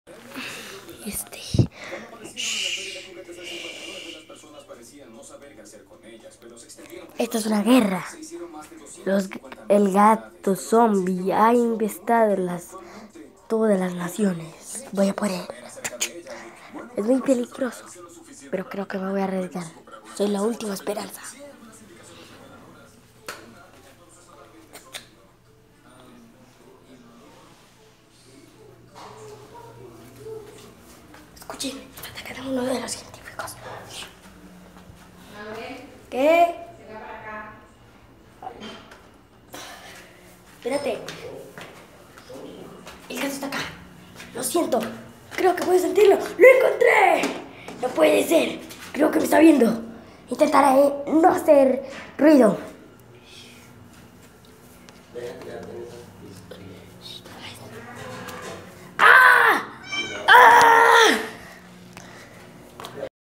niño vs gato
El gato es como um amigo del Niño